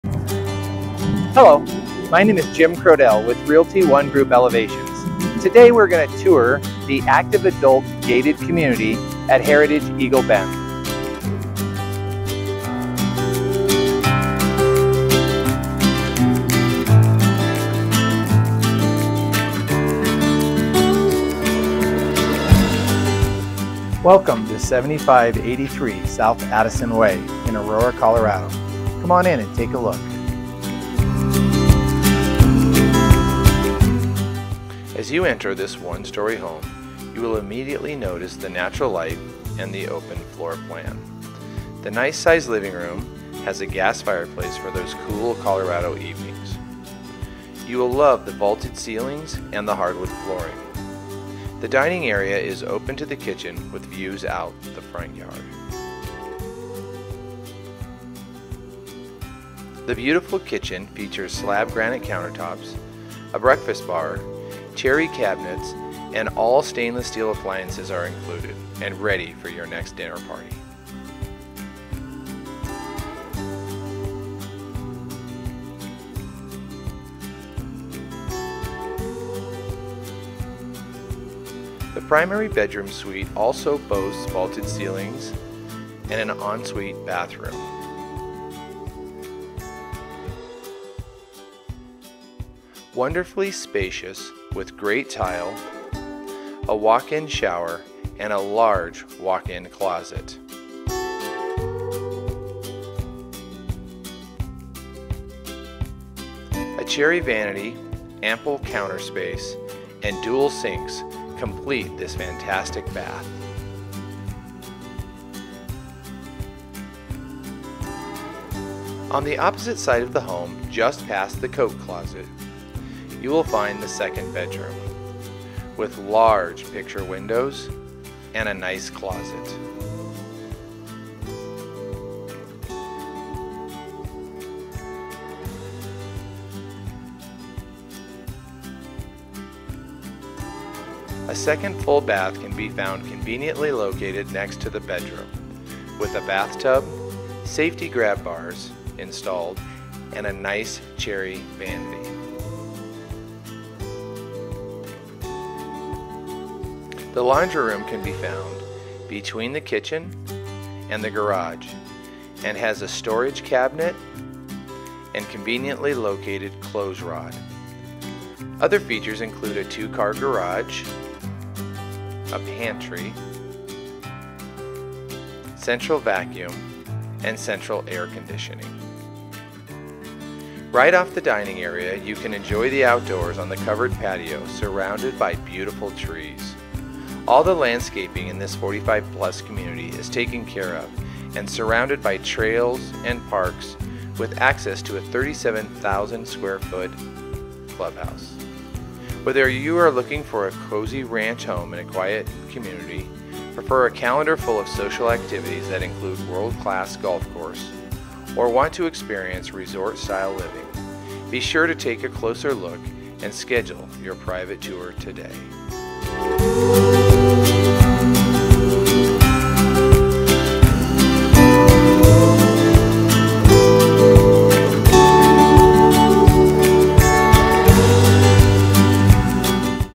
7583 S Addison Way Virtual Tour
Welcome to 7583 S Addison Way in the gated Community of Heritage Eagle Bend in Aurora, Colorado.  Join Jim Krodel with Realty ONE Group Elevations as he takes us on a private tour of this fantastic ranch home!

Whether you are looking for a cozy ranch home in a quiet community, prefer a calendar full of social activities that include a world-class golf course, or want to experience resort-style living, be sure to take a closer look and schedule your private tour today.

To search ALL the homes in the Denver Metro area, go